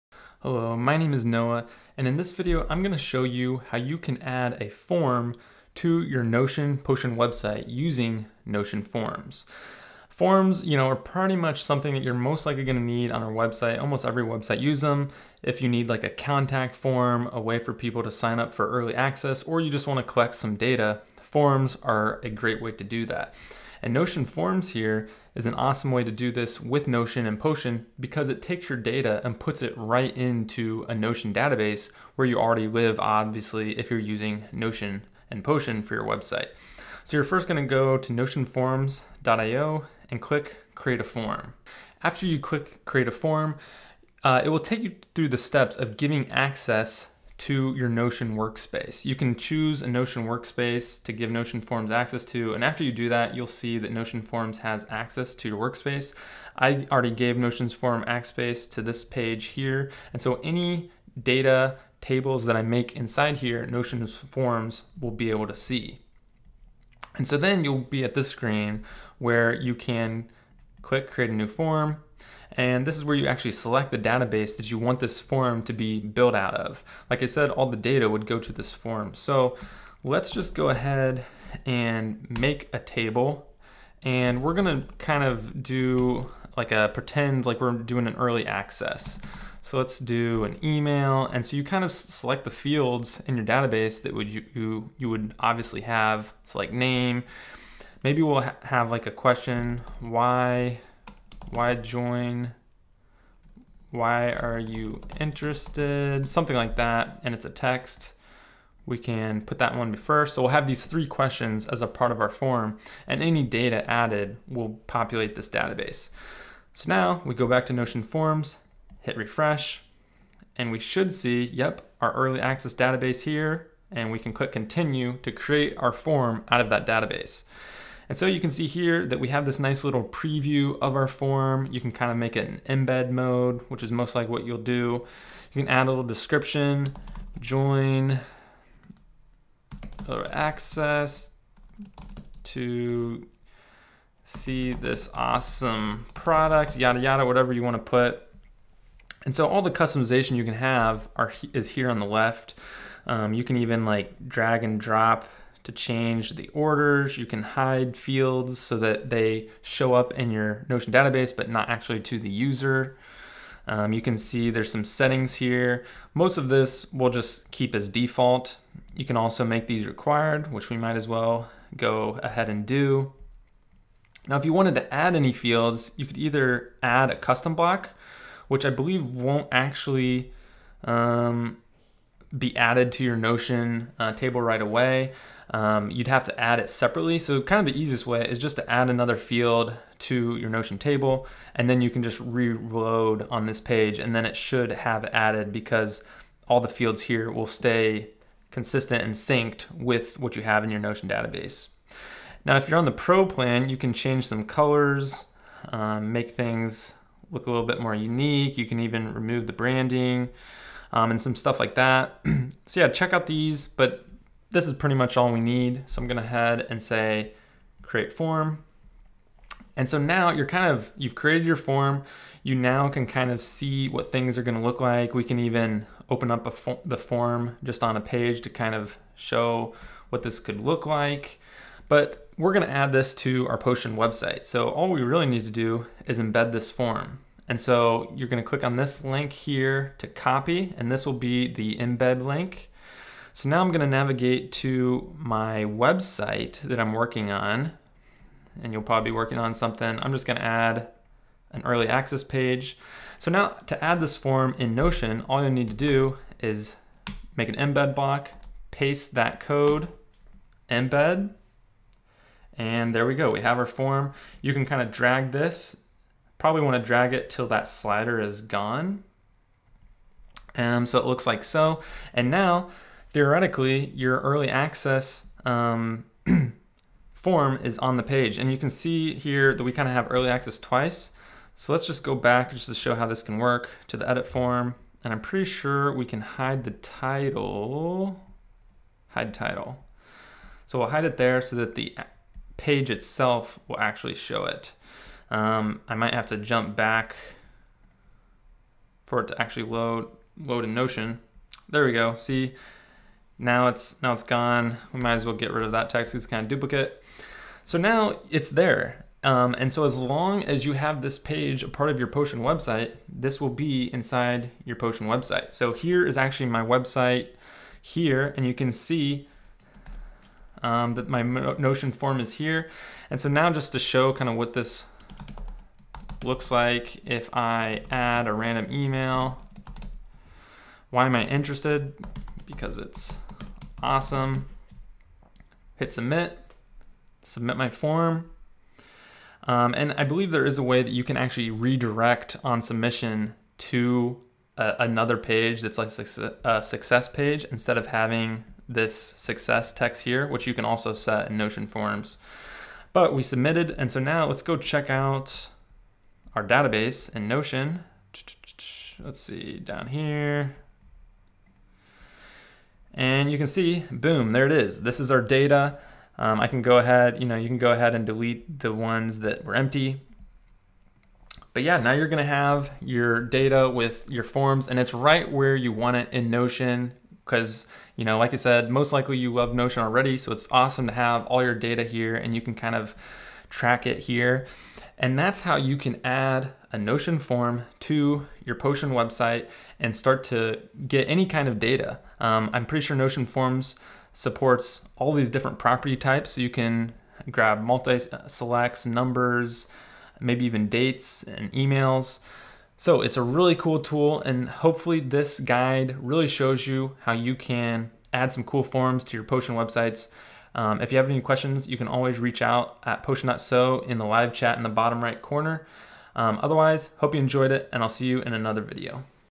How to add a form to your Notion website
Follow along on Twitter to see me build Potion in public!

Potion is a website builder built on top of Notion.